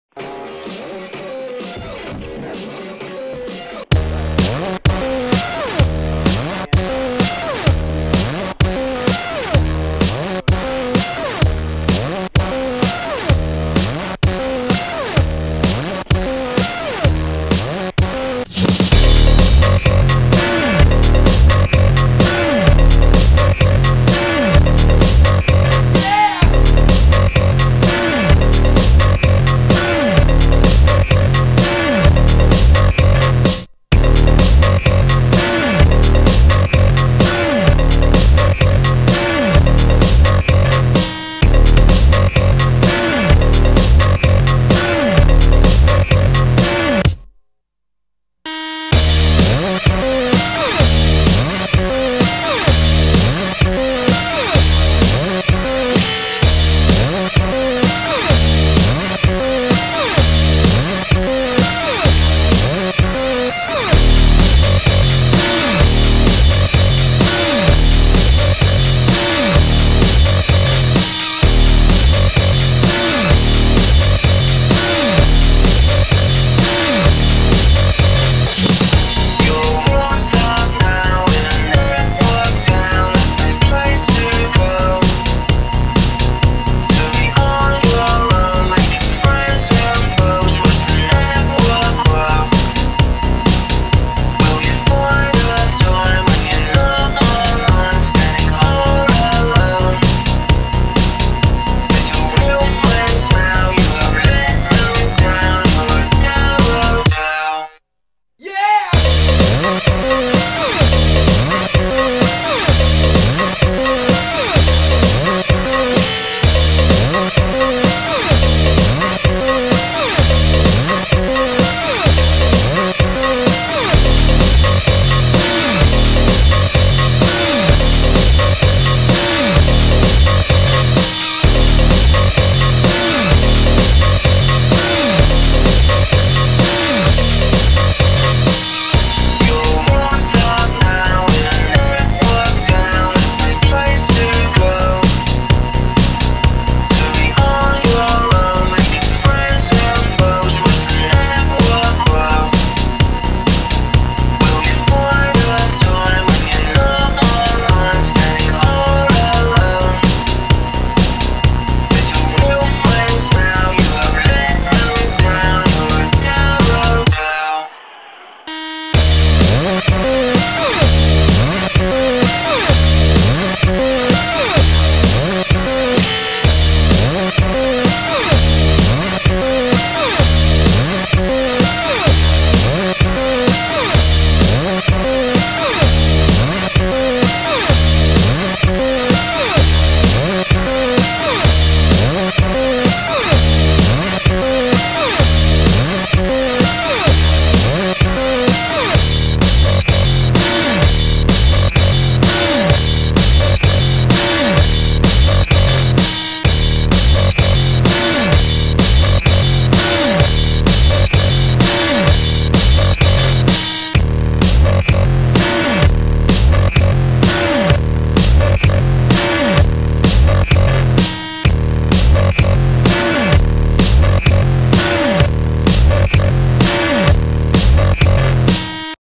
FRC 2010 - Team 1731 Robot Preview - The Sidewinder
Team 1731, Fresta Valley Robotics 2010 Sidewinder Preview. See you at Richmond!